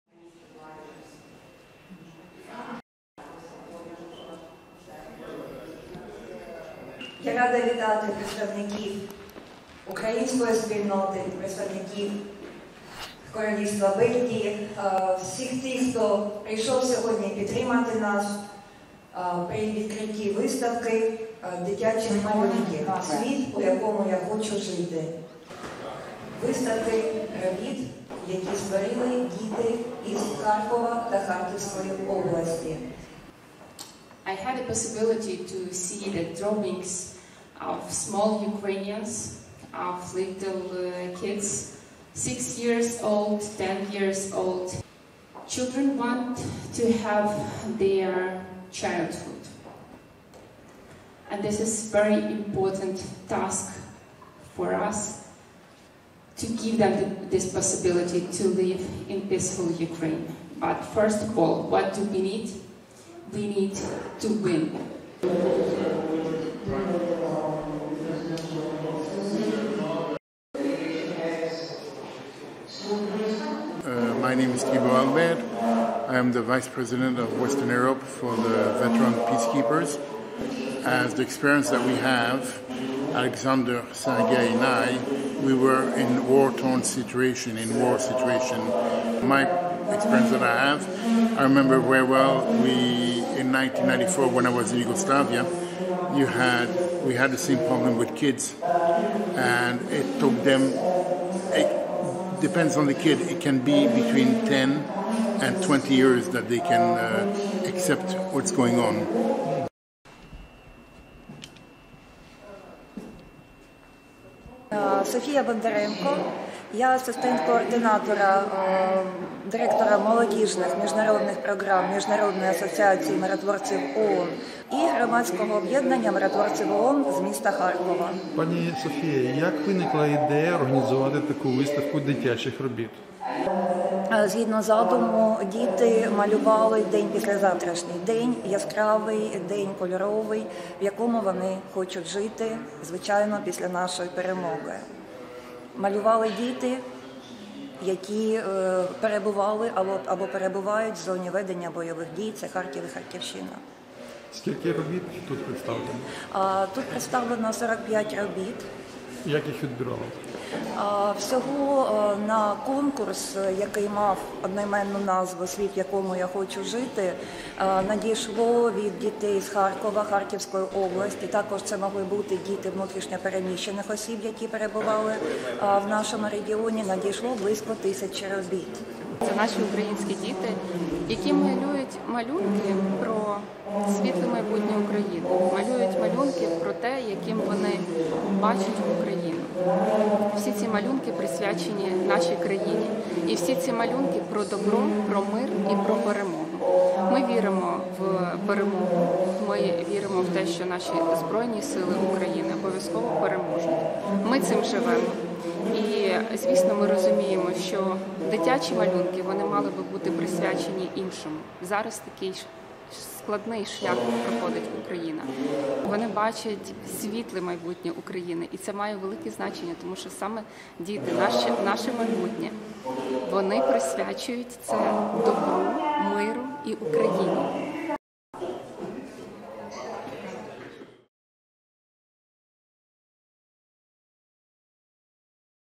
Діти Харківщини мріють про мир на своїй вільній землі
У Брюсселі представили малюнки дітей Харківщини.